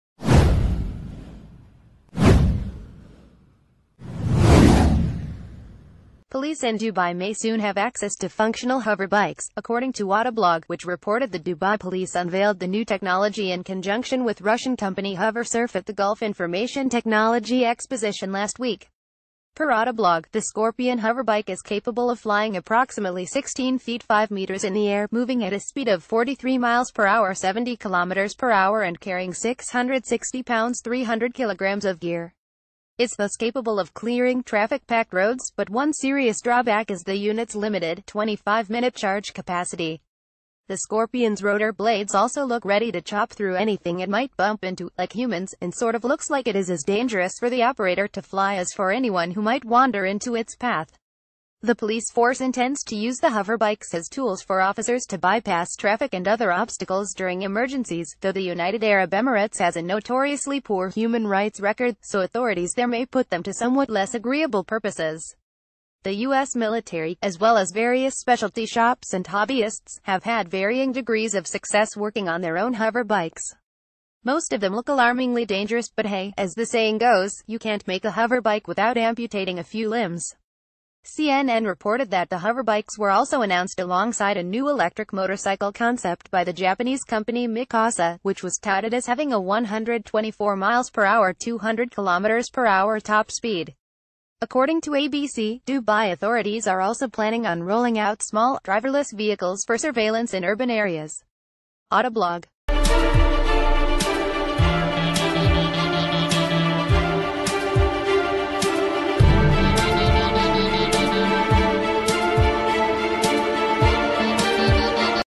Breaking News | Police in dubai are getting real, very dangerous looking hoverbikes
Police in dubai are getting real, very dangerous looking hoverbikes

Police in Dubai may soon have access to functional hoverbikes, according to AutoBlog, which reported...